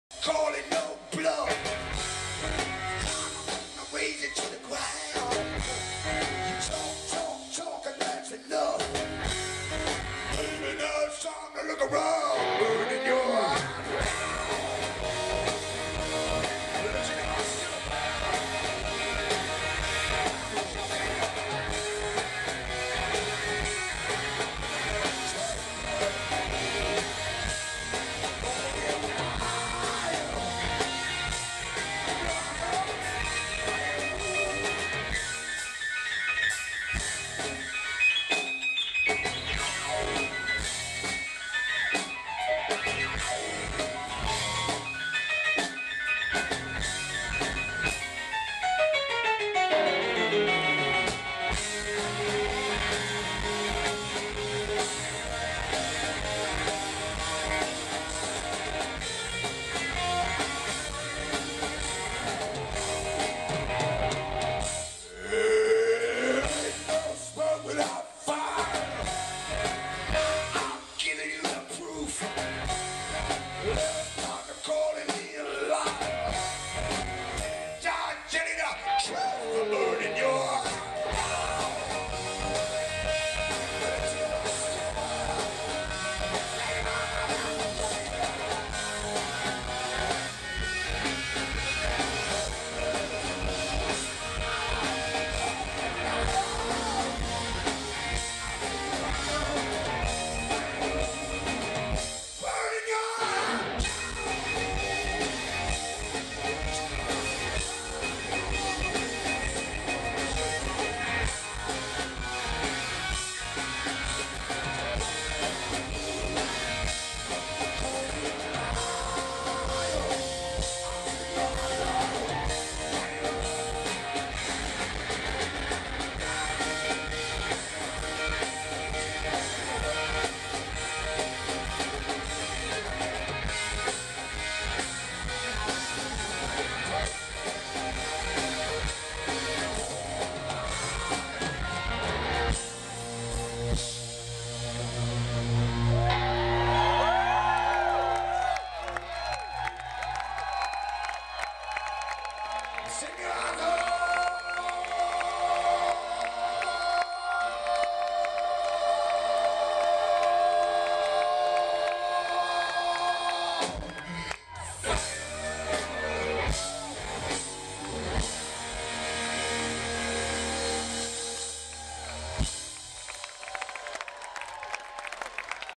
Jim Jones Review plays Bumbershoot.MP4
Jim Jones Review plays the Mural Amphitheater at Bumbershoot on Sunday September 4, 2011.